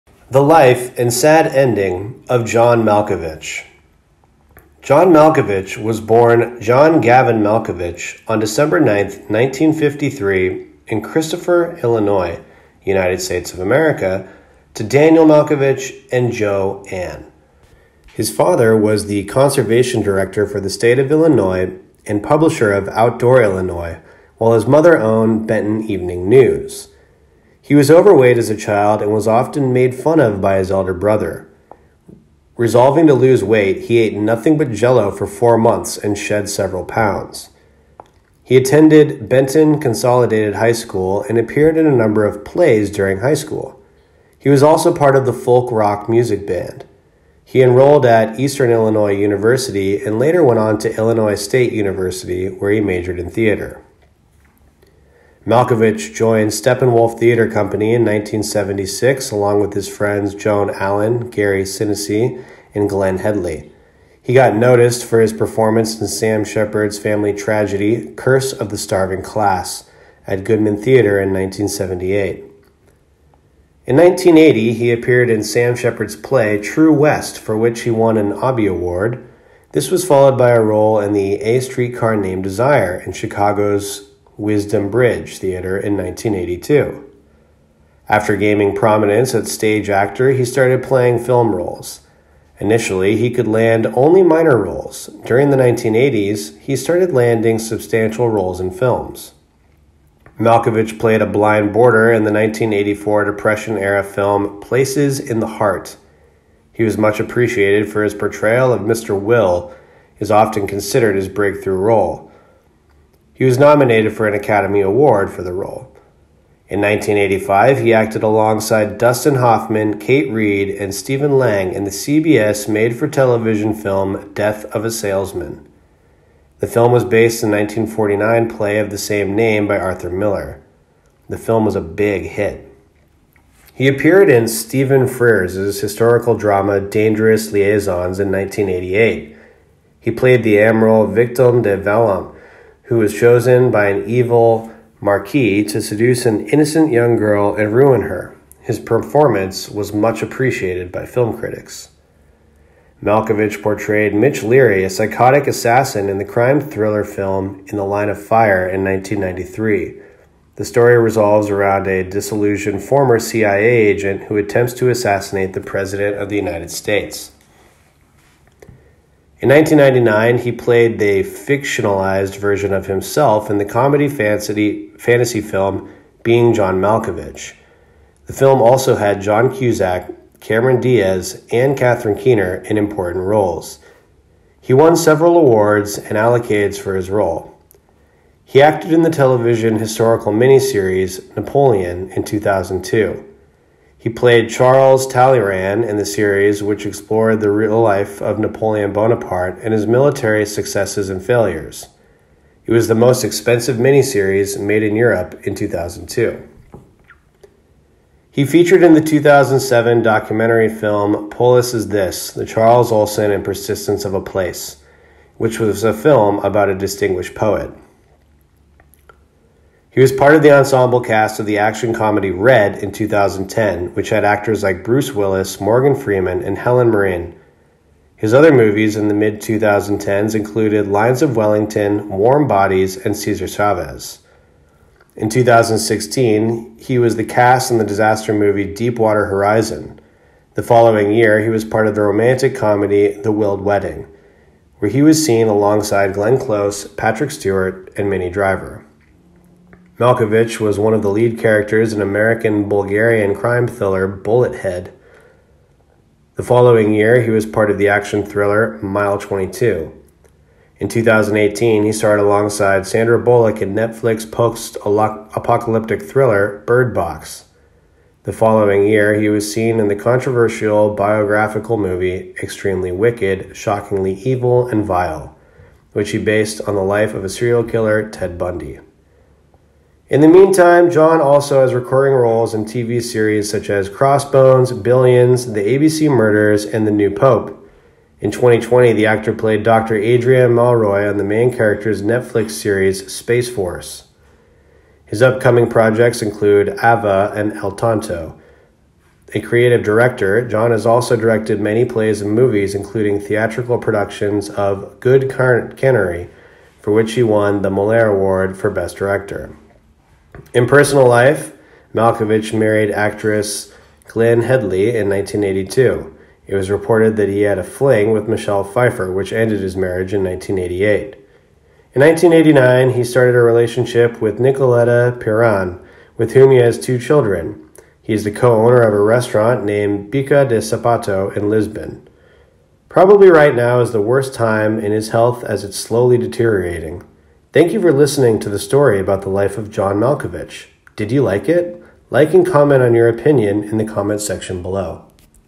The Life and Tragic Ending of John Malkovich
John Malkovich was born John Gavin Malkovich on December 9, 1953, in Christopher, Illinois, U.S, to Daniel Malkovich and Joe Anne. His father was the conservation director for the State of Illinois and the publisher of ‘Outdoor Illinois,’ while his mother owned ‘Benton Evening News.’